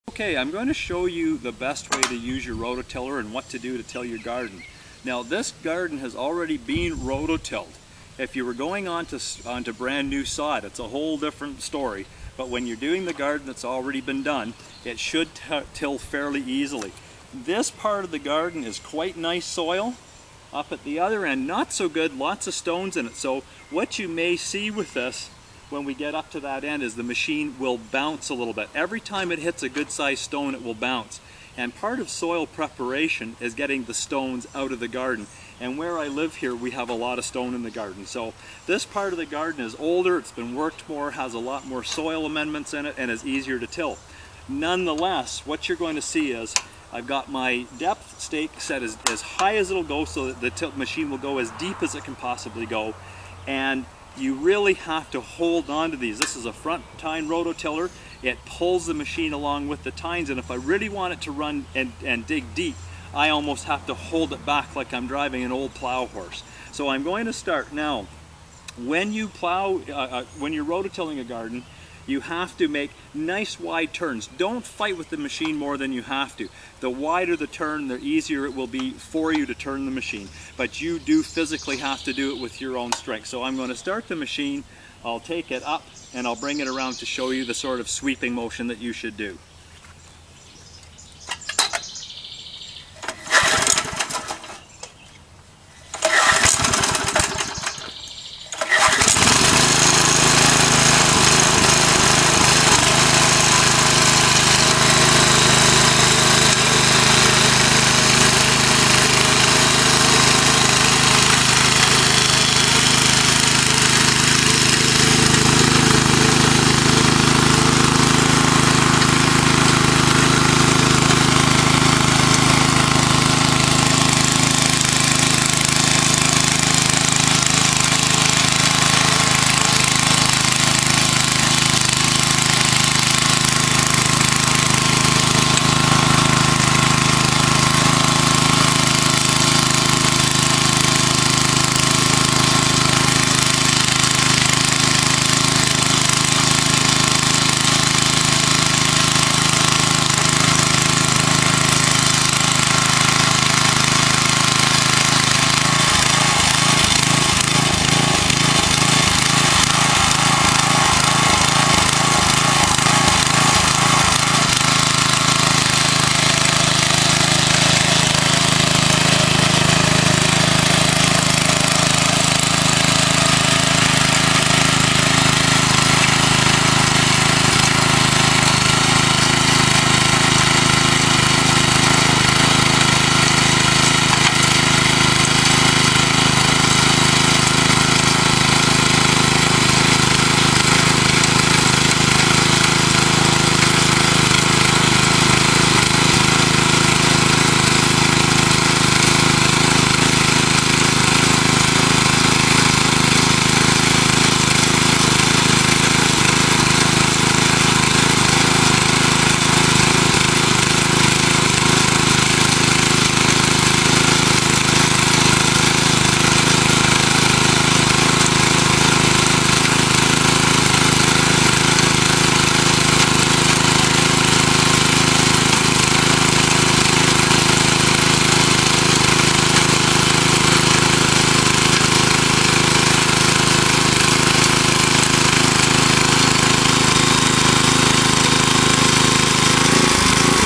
SDV_0015.MP4
Brian a.k.a. More Gopher Cake shows you how to run a front tine rototiller